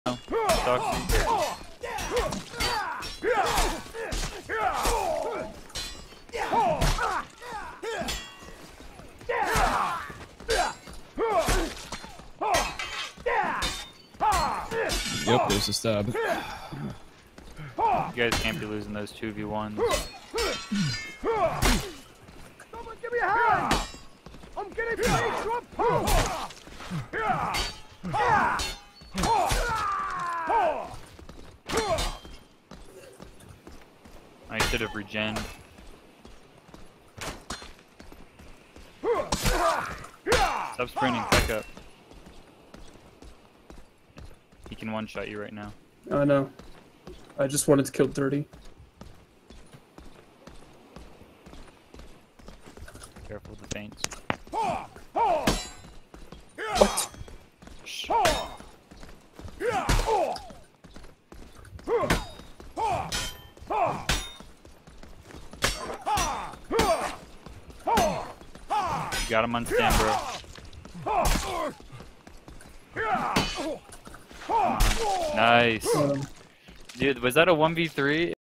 Vq vs. GKG | MonoXtr Clutch
Impressive 1v3 by MonoXtr